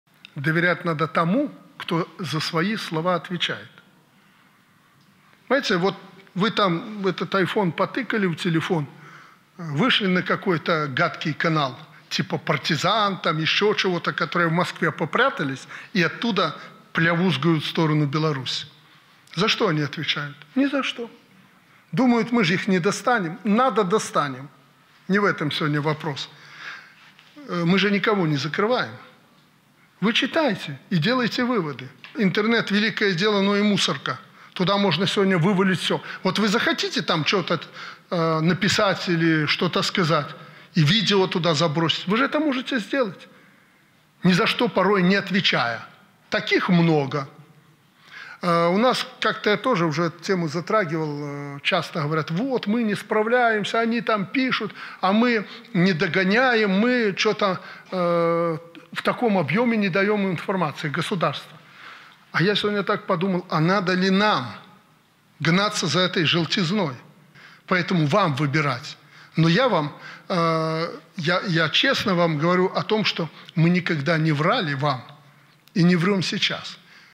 Лукашенко: в Москве попрятались и "плявузгают" в сторону Беларуси. Думают, мы их не достанем.
Последние новости. Александр Лукашенко во время своего визита в Бобруйск высказался по поводу непроверенных источников информации.

"Доверять надо тому, кто за свои слова отвечает. Вышли вы на какой-то из гадких каналов, которые в Москве попрятались и оттуда "плявузгают" в сторону Беларуси, - за что они отвечают? Ни за что. Думают, мы же их не достанем. Если надо - достанем. Не в этом сегодня вопрос. Мы же никого не закрываем. Вы читайте и делайте выводы. И верьте тому, с кого потом спросить можно. А надо ли нам гнаться за этой желтизной.  Но я честно вам говорю, что мы никогда не врали вам и не врем сейчас".